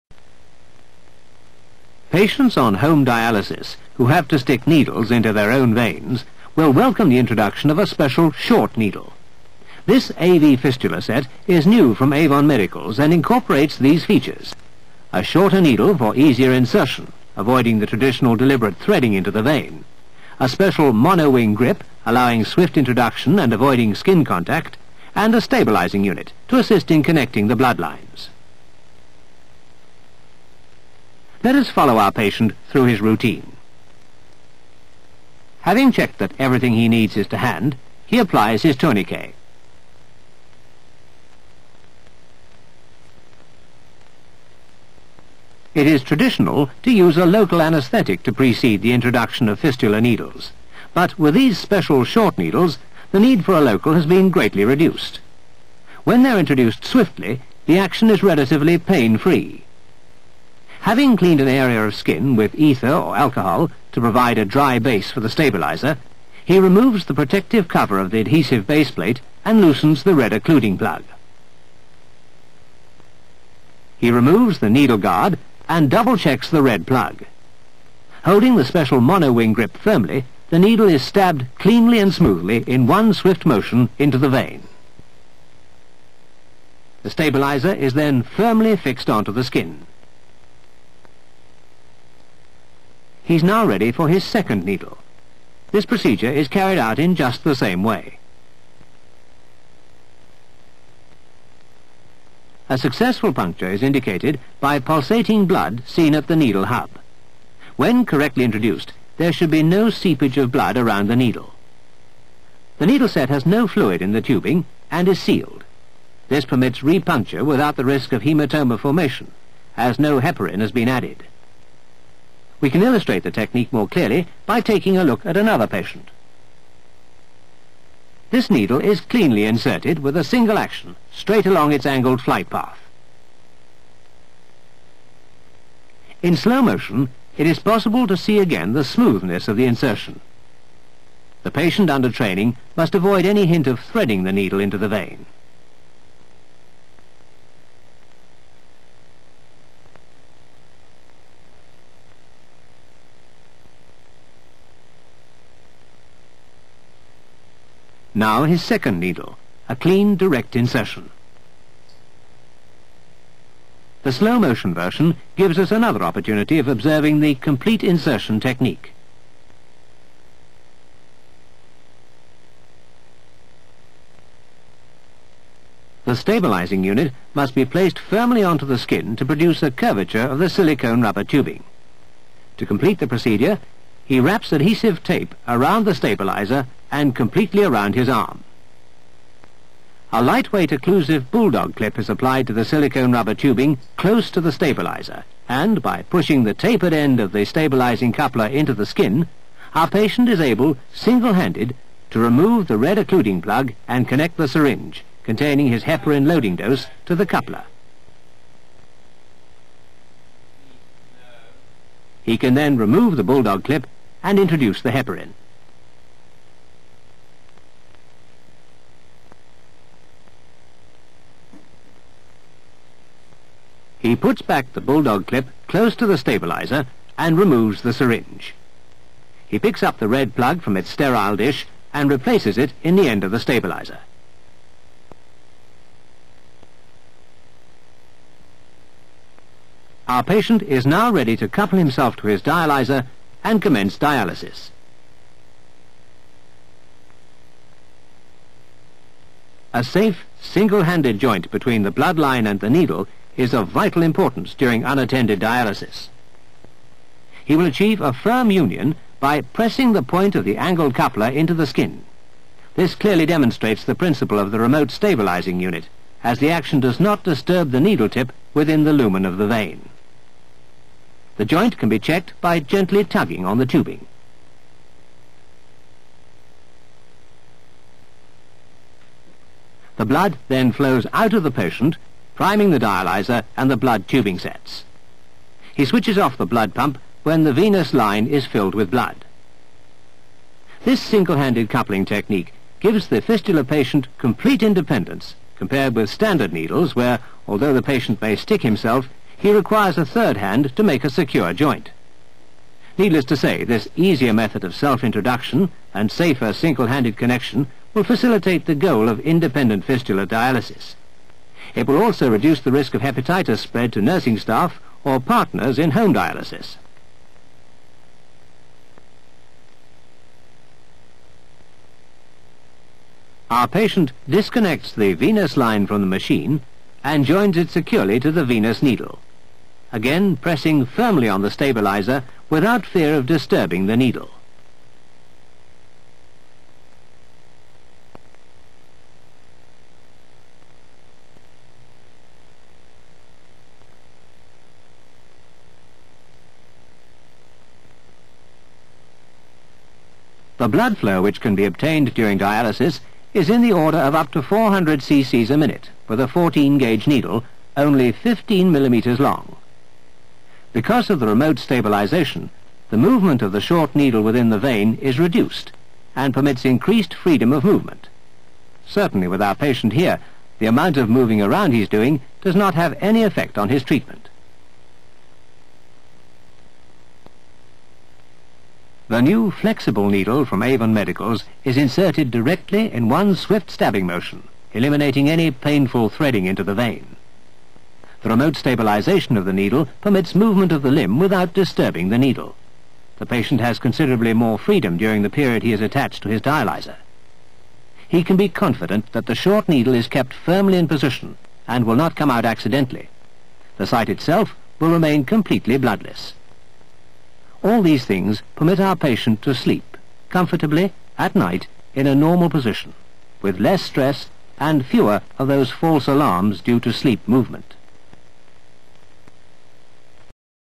TECHNIQUE OF SELF HEMODIALYSIS
filmed in 1969 this historic video demonstrates the principles of self dialysis using an AV fistula  developed by Stanley Shaldon MD at the National Kidney center in London